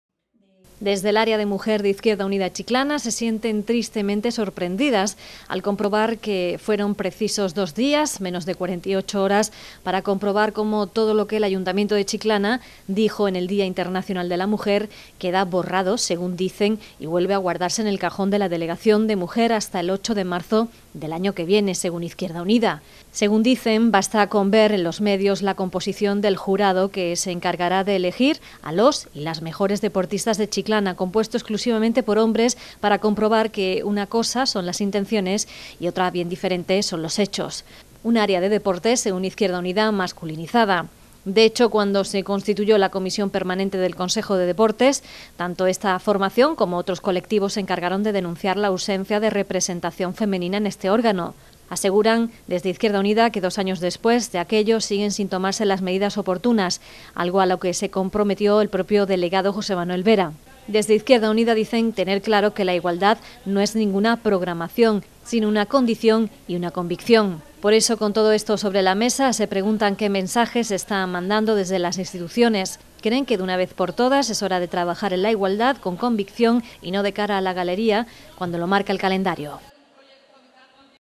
BALANCE NEGATIVO
Después de una semana de reflexión tras la intensa jornada reivindicativa vivida el pasado 8 de marzo con motivo del Día Internacional de la Mujer, Izquierda Unida Chiclana considera que fue histórica por el masivo seguimiento con que contó y por el punto de inflexión que supuso en el camino hacia la igualdad real entre hombres y mujeres. Sin embargo, esto contrasta con el posicionamiento del Gobierno local